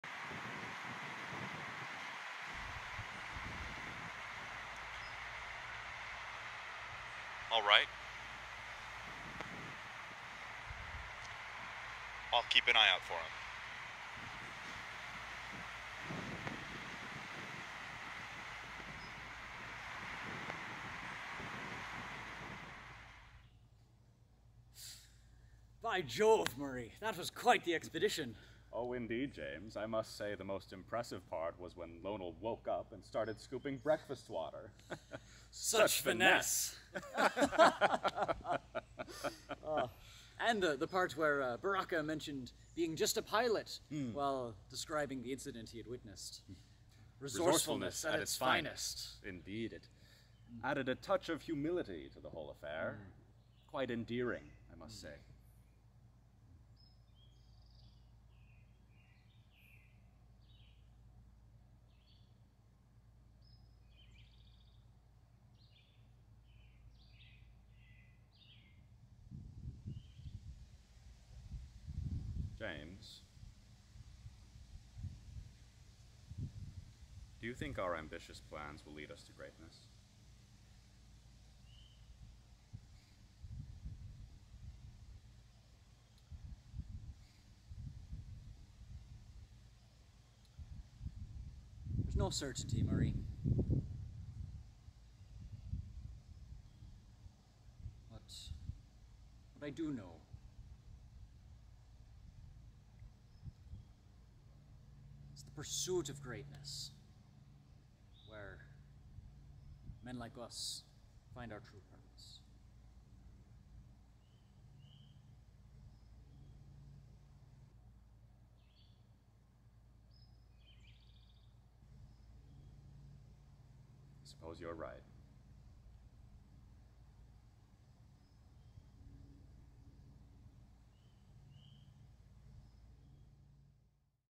Forest Brothers
Two brothers, James and Murray, enjoy each other's company in the forest.